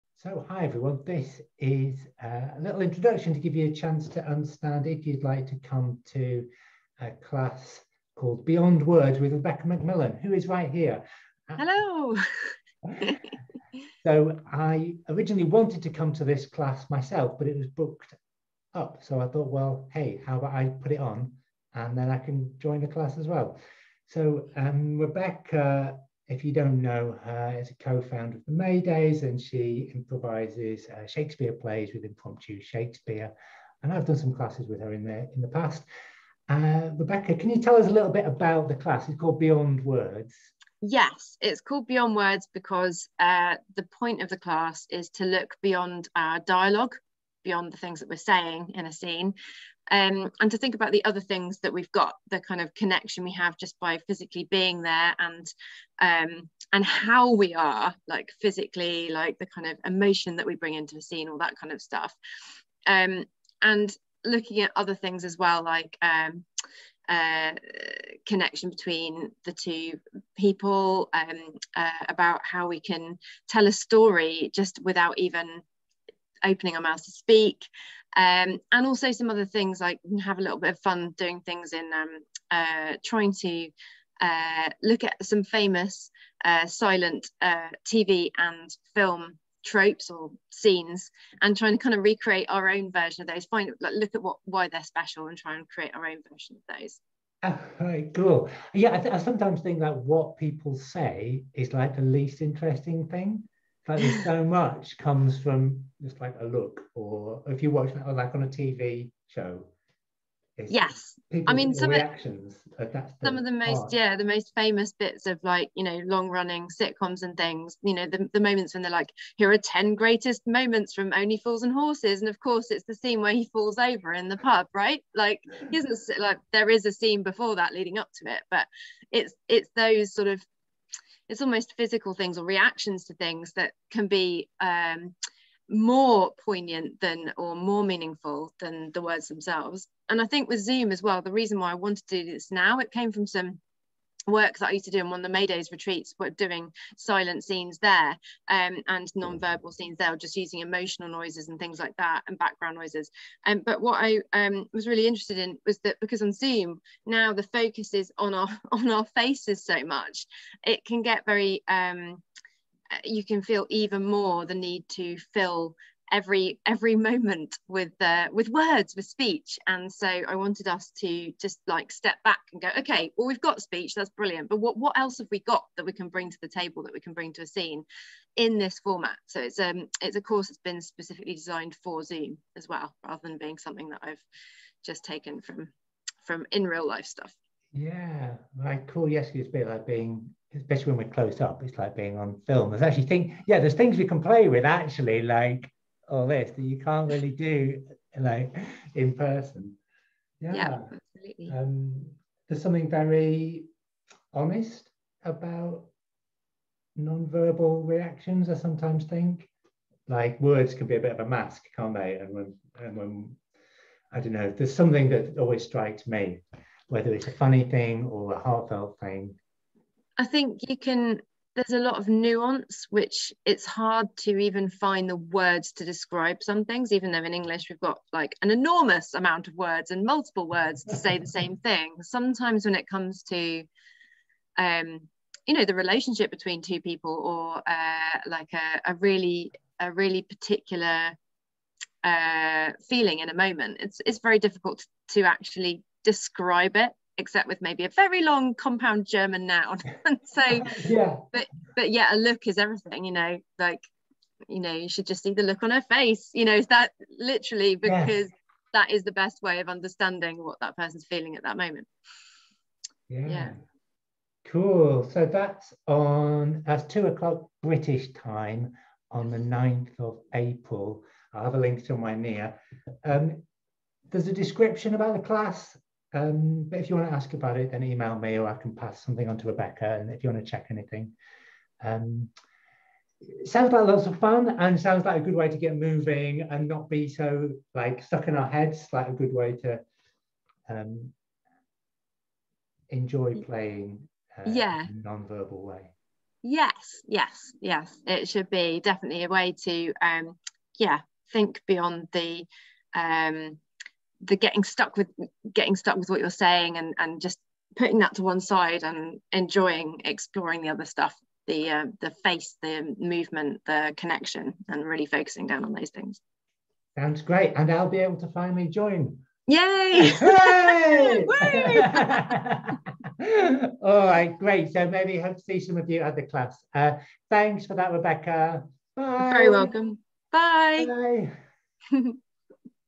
Beyond Words improv class at PlayConnect.co.uk
Improvising without words with Rebecca MacMillan of The Maydays and Impromptu Shakespeare.  A quick chat and describing the class on 9th April.

Only Fools and Horses is a British comedy.  The moment Rebecca mentions is here :-)